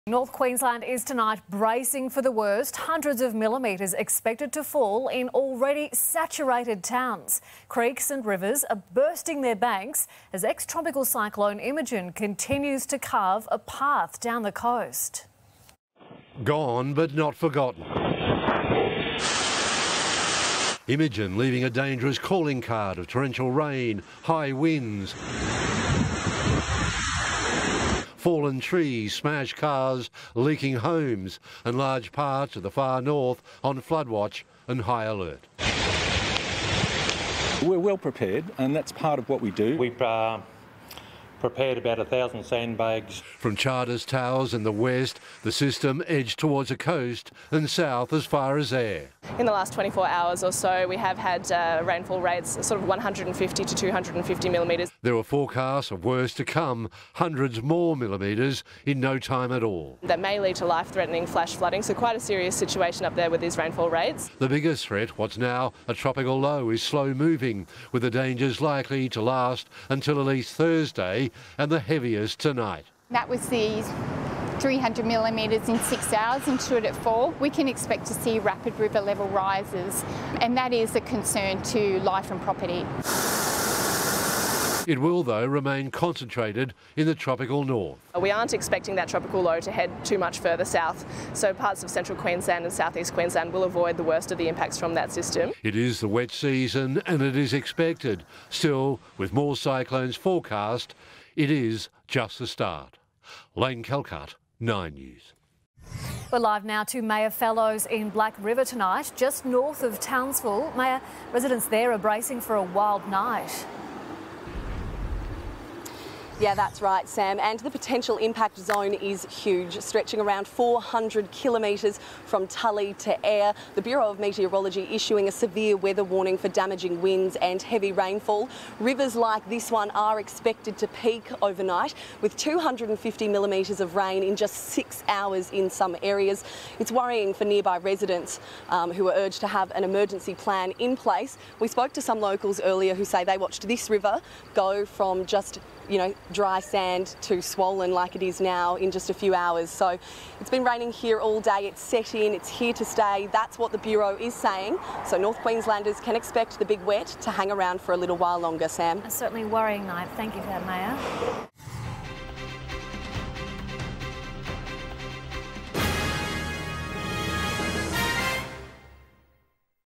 North Queensland bracing for worst of Cyclone Imogen | 9 News Australia
Heavy rain and bursting creek and riverbanks are expected to lash North Queensland as residents brace for the worst of Cyclone Imogen yet.